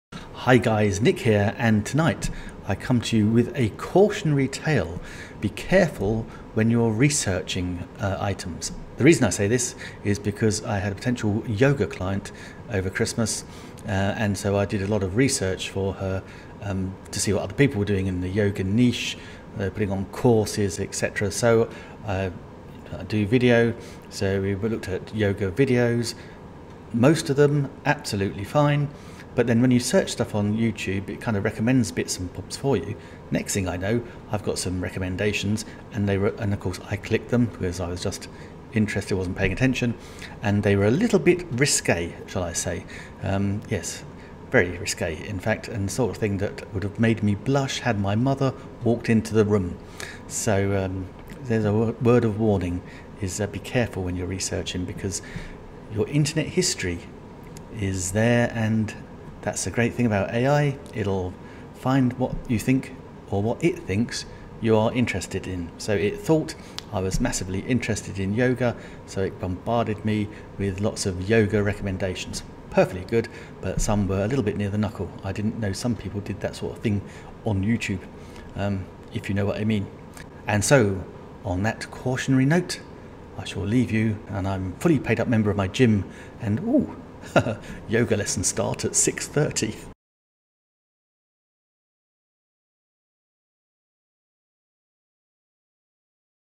Keyword research for Yoga | Pitfalls of researching Yoga keywords
Speaker 1:          00:00          Hi Guys, Nick here and tonight I come to you with a cautionary tale. Be careful when you're researching a items. The reason I say this is because I had a potential yoga client over Christmas and so I did a lot of research for her to see what other people were doing in the Yoga Niche, putting on courses, etc. So I do video, so we will looked at yoga videos, most of them absolutely fine, but then when you search stuff on youtube but it kind of recommends bits and pops for you. Next thing I know I've got Some recommendatIons and they were. And of course I click them, whereas I was just interested, wasn't paying attention and they were a little bit risque, shall I say. Um, yes, very risky in fact, and sort of thing that would have made me blush had my mother walked into the room.

Speaker 1:          00:50          So, um, there's a word of warning is that be careful when you're researching because your internet history is there. And that's the great thing about ai. It'll find what you think or what it thinks you are interested in. So it thought I was massively interested in yoga, so bombarded me with lots of yoga recommendations, perfectly good. But some were a little bit near the knuckle. I didn't know some people did that sort of thing on youtube, if you know what I mean. And so on that cautionary note, I shall leave you and I'm fully paid up member of my gym and oh, yoga lesson starts at 6:30.
xxxxxxxxxxxxxxxxxxxxxxxxxxxxxxxxxxxxxxxxxxxxxxxxxxxxxxxxxxxxxxx
xxxxxxxxxxxxxxxxxxxxxxxxxxxxxxxxxxxxxxxxxxxxxxxxxxxxxxxxxxxxxxx

Nick Wood
Video Marketing and Social Media Expert